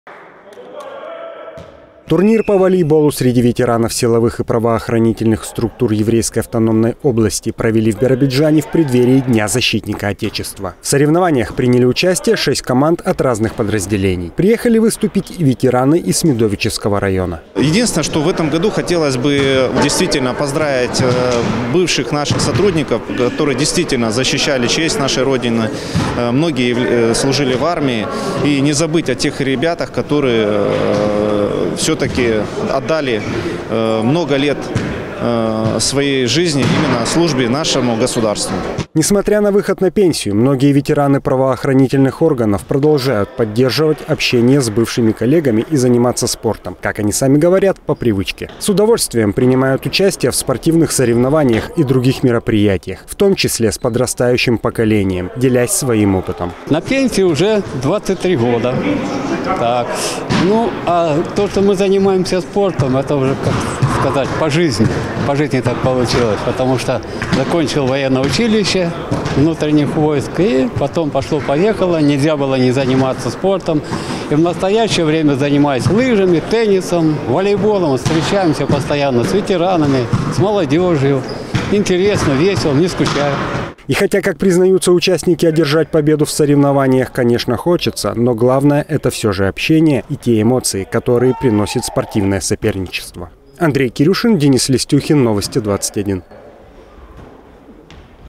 Турнир по волейболу среди ветеранов силовых структур прошел в Биробиджане
Турнир по волейболу среди ветеранов силовых и правоохранительных структур Еврейской автономной области провели в Биробиджане в преддверии Дня защитника Отечества. В соревнованиях приняли участие шесть команд от разных подразделений. Приехали выступить и ветераны из Смидовичского района.